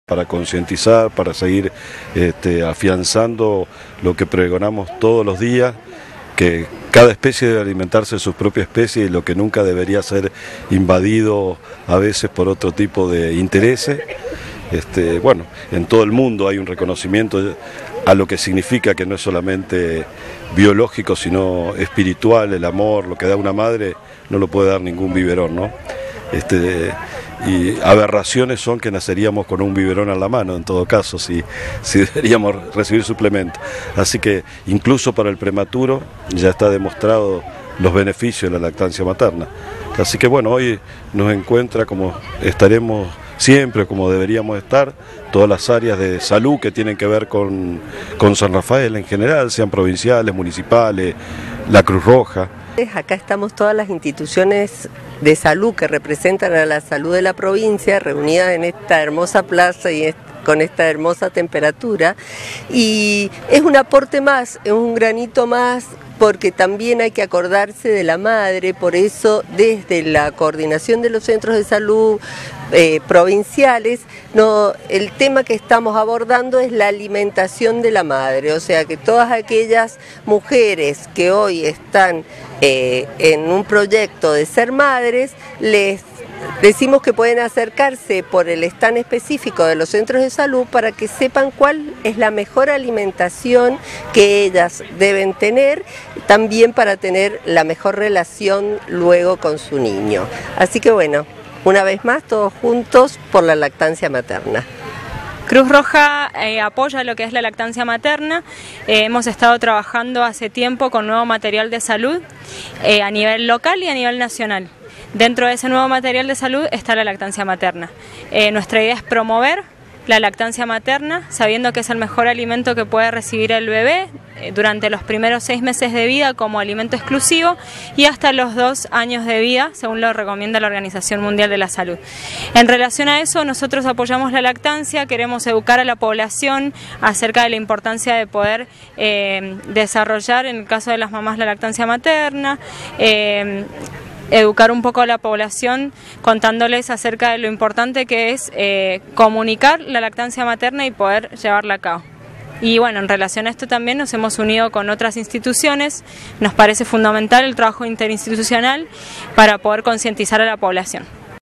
PLAZA FRANCIA, EL ESCENARIO
Este domingo pasado se realizo,  en Plaza Francia una teteada, donde se les explico a mamas y público en general sobre la gran importancia de la leche materna, ademas de sostener los derechos de hacerlo en lugares públicos